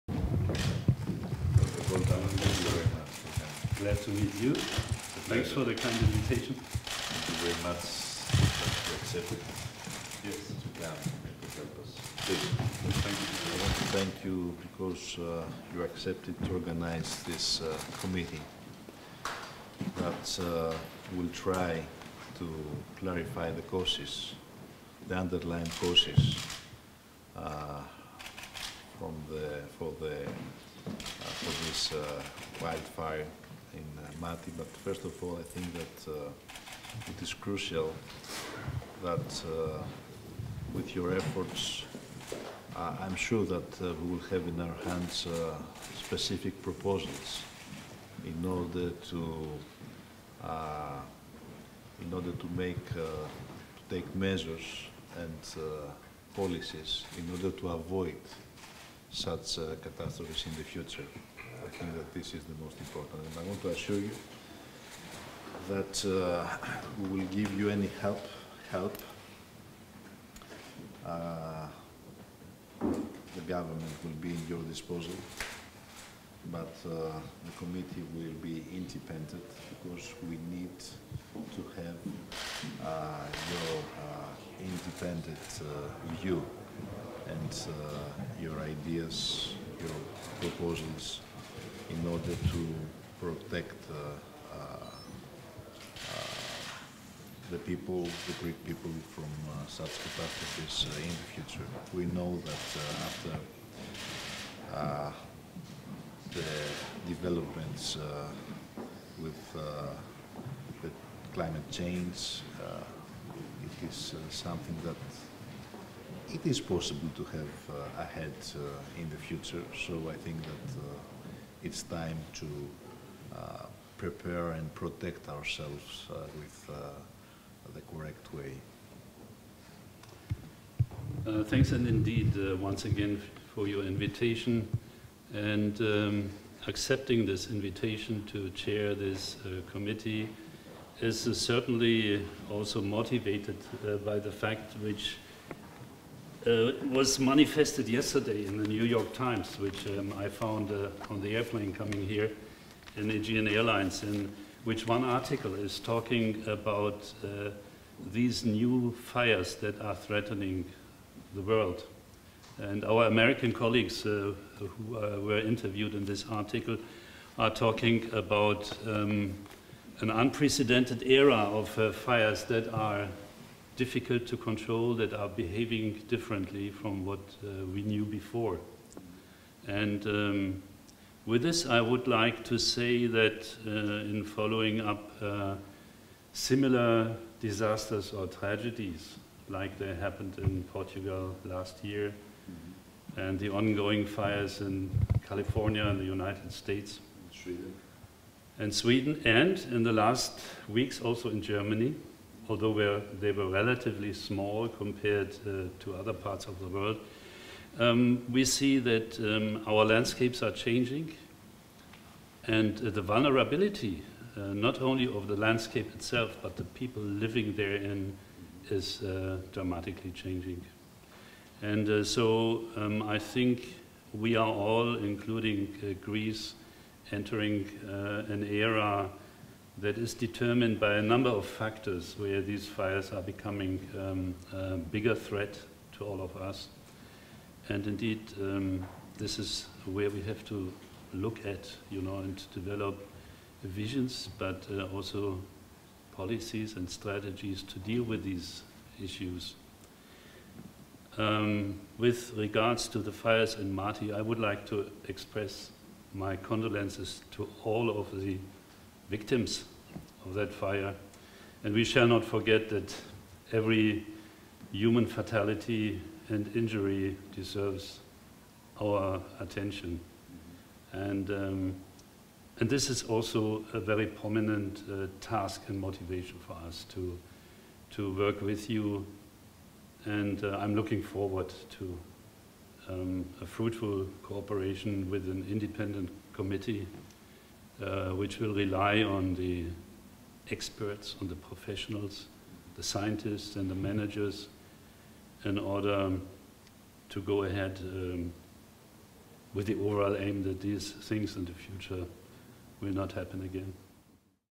Συνάντηση με τον καθηγητή Δρ. Γιόχαν Γκέοργκ Γκόλνταμερ
Συνάντηση του Πρωθυπουργού Αλέξη Τσίπρα με τον καθηγητή Δρ. Γιόχαν Γκέοργκ Γκόλνταμερ, Διευθυντή του Παγκόσμιου Κέντρου Παρακολούθησης Πυρκαγιών (GFMC), Μέγαρο Μαξίμου, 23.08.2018.